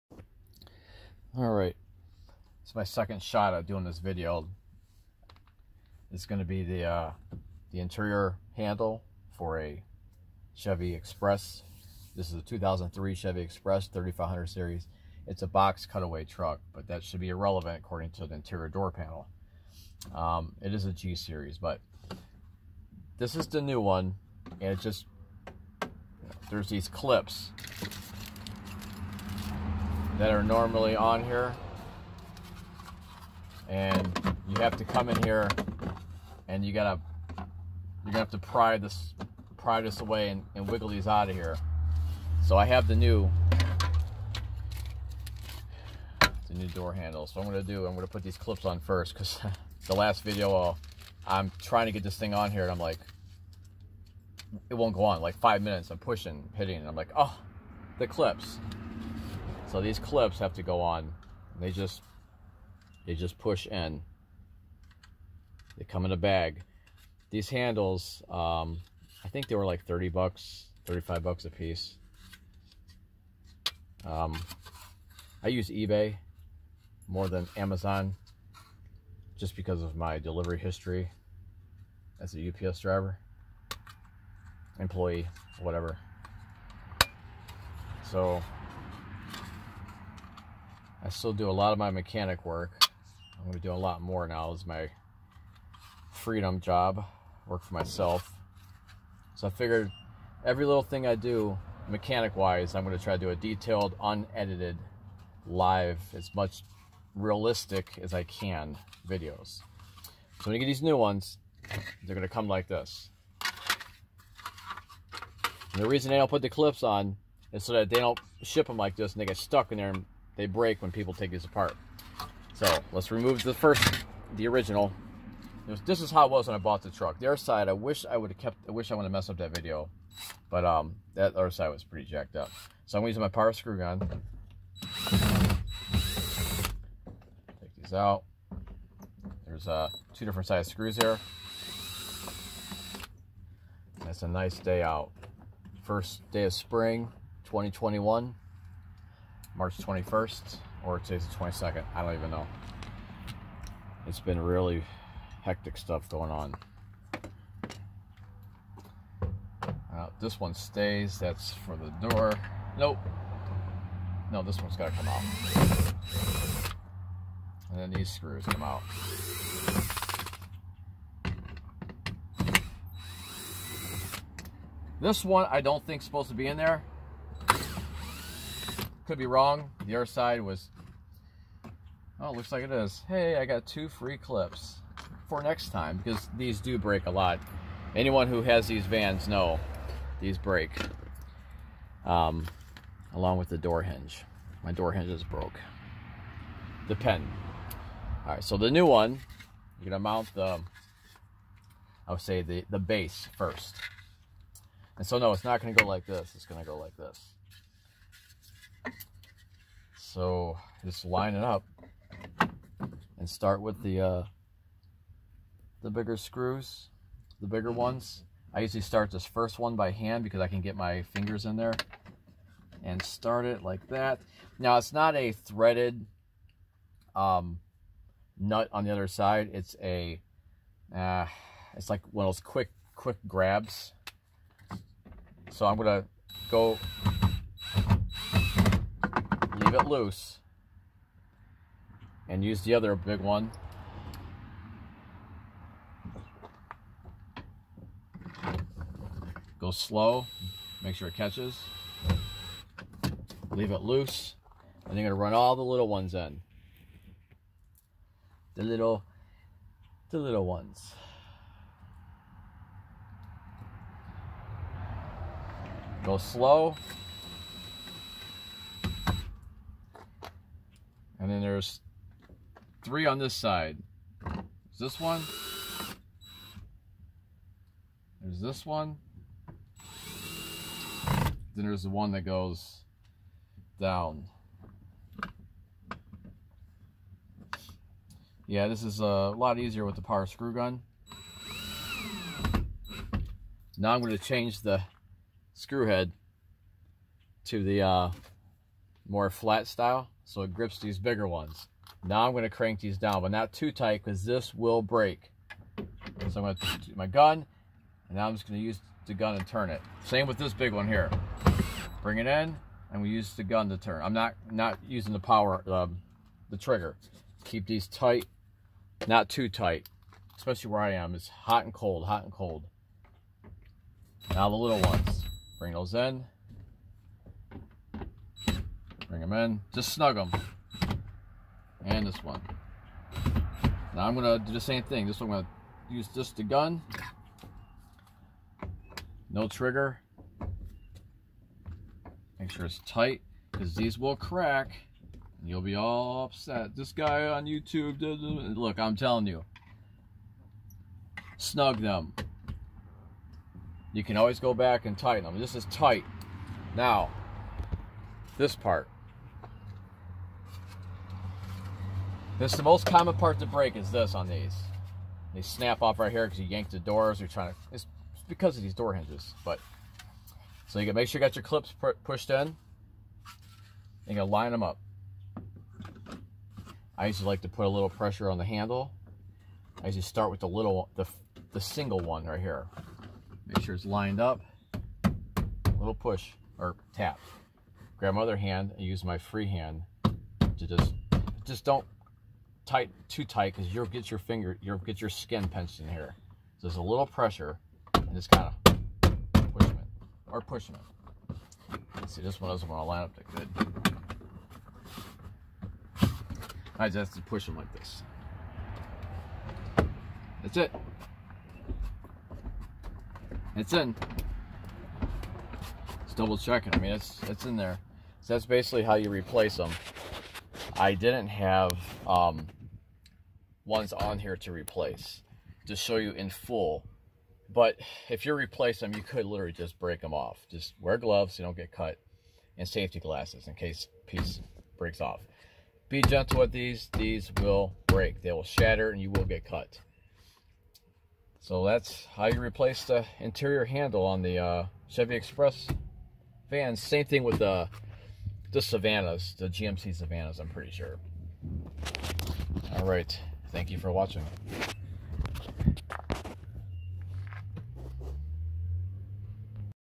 2003 Chevrolet Express Van interior door handle replacement
2003 Chevrolet Express G3500 Cut A Way Box Van Truck interior door handle replacement

First Day of spring 3/21/21